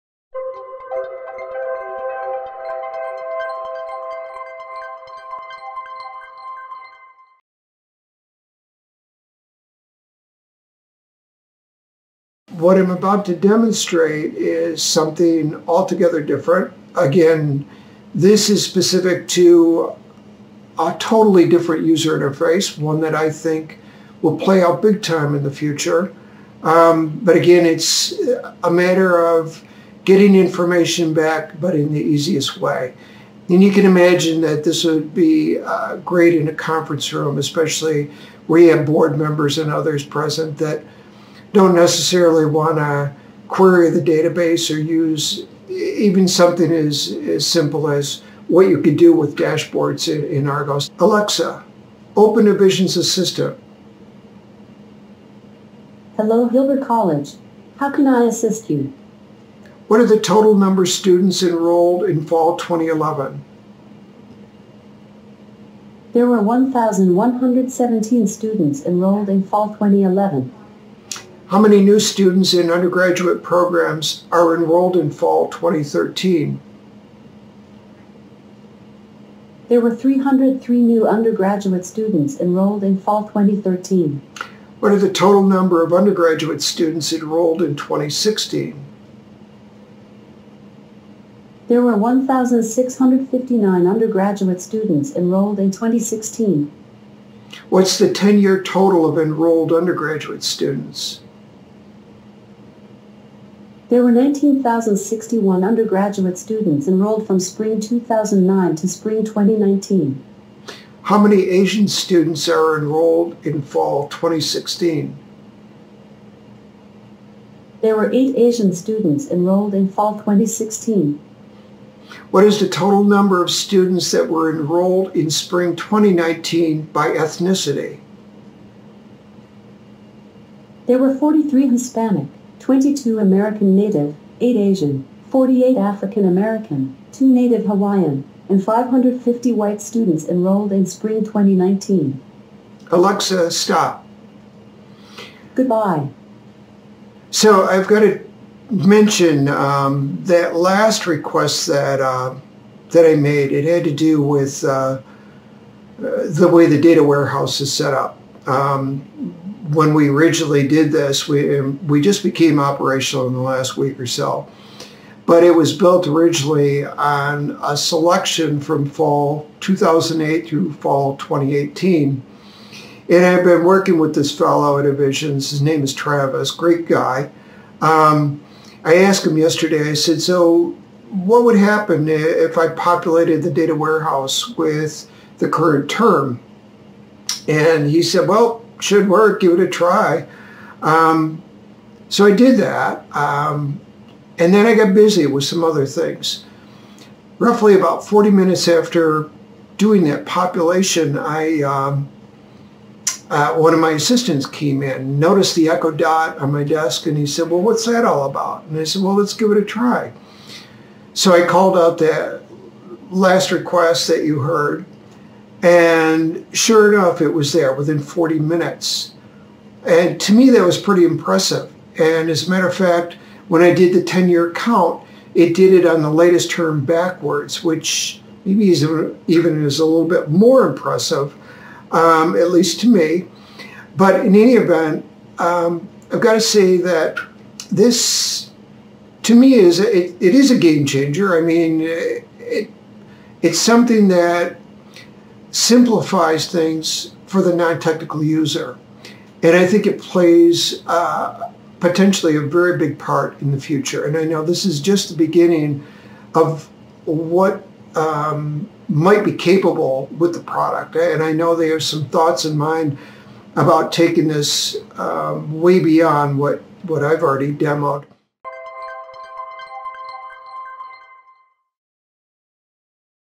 Hilbert College Demos EASE for Amazon Alexa
Michael Murrin, VP for Information Services at Hilbert College, gives a first-hand demonstration of using EASE for Amazon Alexa to provide key institutional data. He also discusses potential scenarios for how this type of reporting would be used.